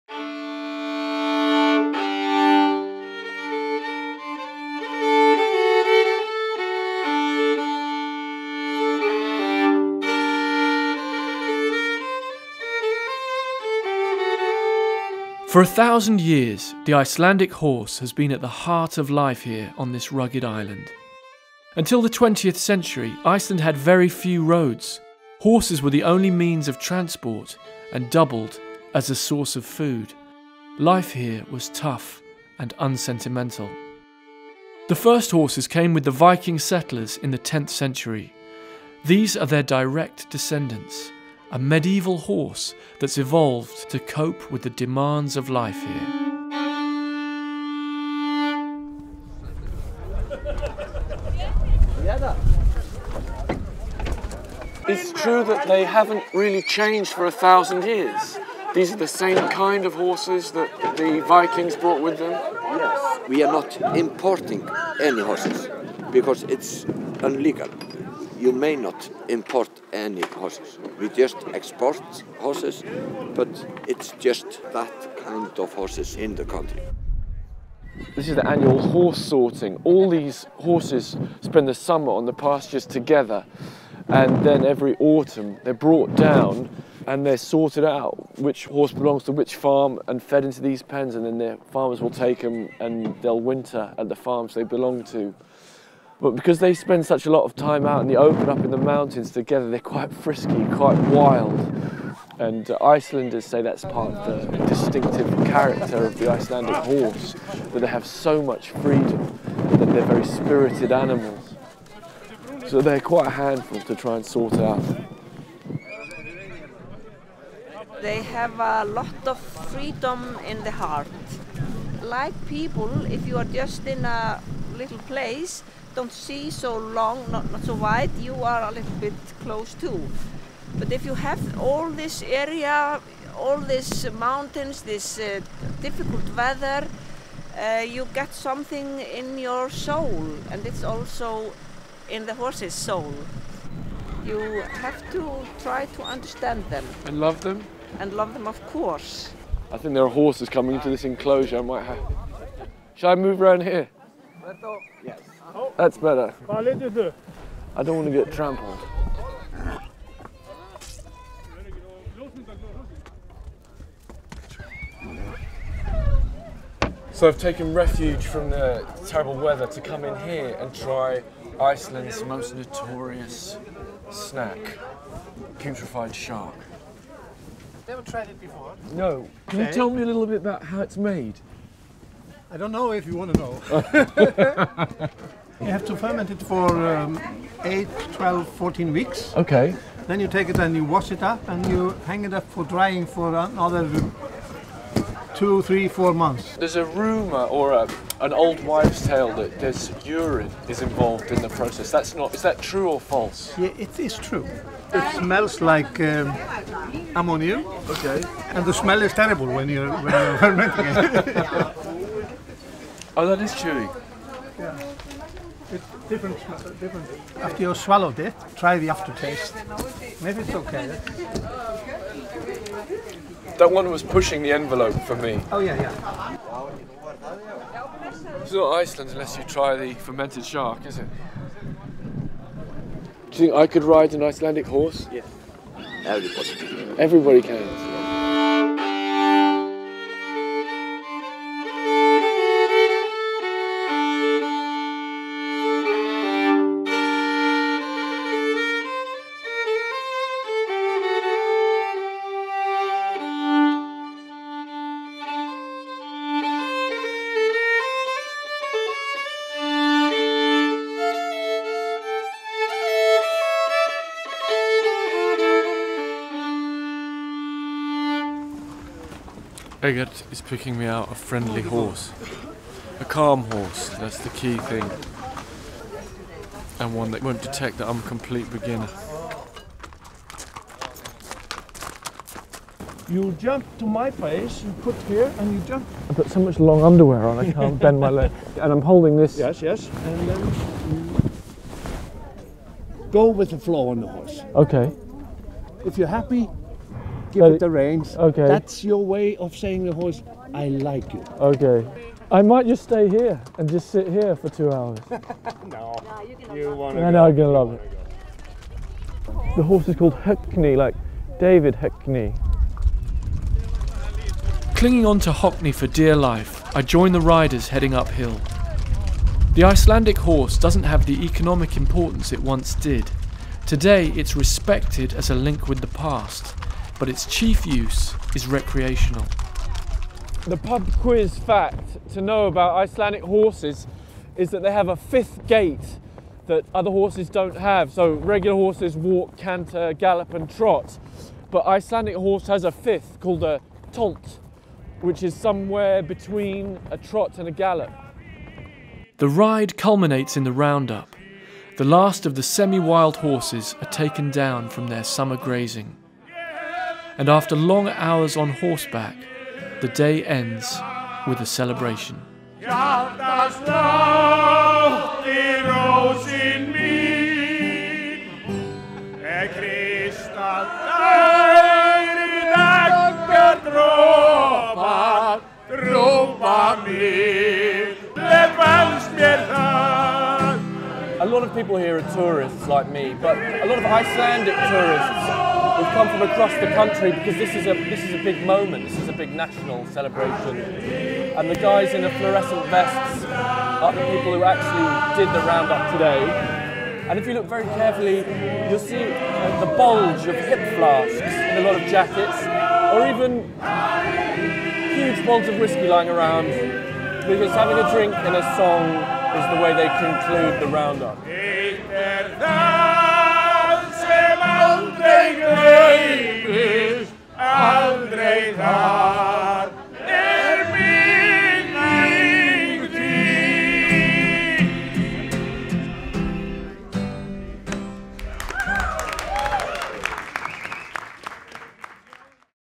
Horse riding in Iceland with Marcel Theroux
Reykjavik may be at the cutting edge of cool but in rural Iceland, ancient traditions are still observed. Travel writer Marcel Theroux heads to the north of the country for the annual horse round-up, goes riding across the windswept pastures and joins farmers for a drink and a sing-along